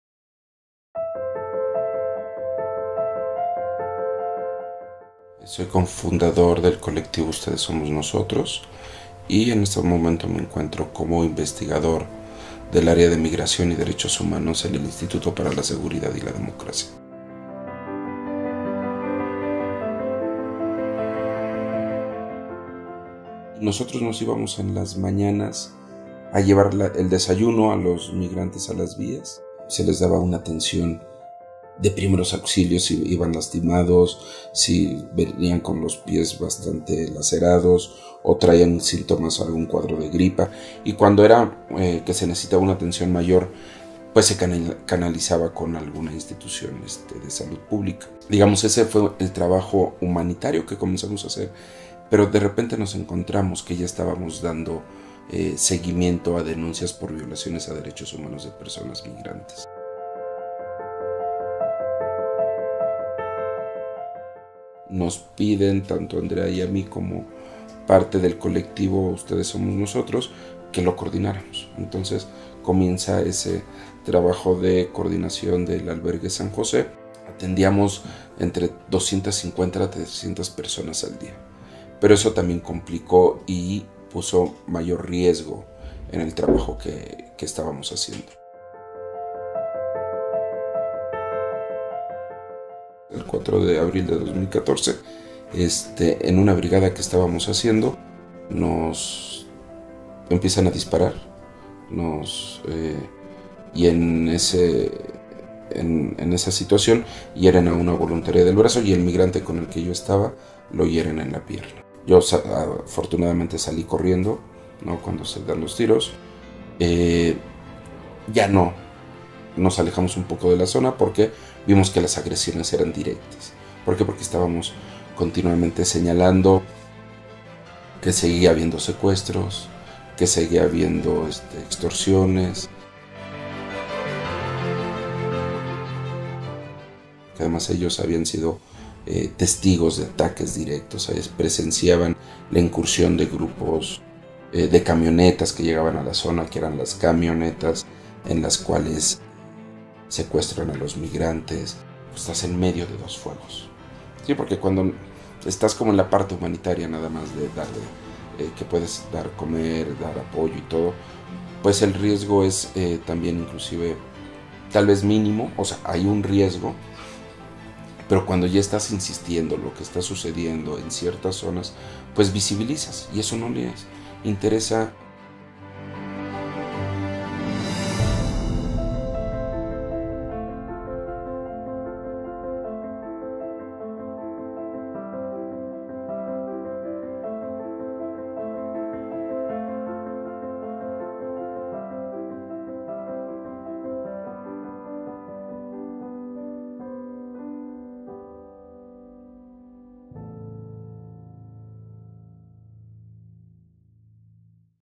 Jorge Andrade
CMDPDH.

Ustedes Somos Nosotros surge en julio de 2012 en respuesta a acontecimientos como el cierre del albergue San Juan Diego, en Lechería, Estado de México, y el descarrilamiento del tren que dejó varados a cientos de personas migrantes en el sur de México. El Colectivo proporciona asistencia humanitaria, asesoría y acompañamiento por violaciones a derechos humanos de personas migrantes en la ruta del Estado de México. Sus cofundadores Andrea González y Jorge Andrade han sido objeto de amenazas, hostigamientos, allanamiento y robo de información, éste último sufrió un ataque en abril de 2014 en donde resultaron heridas dos personas por arma de fuego. En noviembre de 2014, dos voluntarios del Colectivo, Adrián y Wilson, fueron asesinados.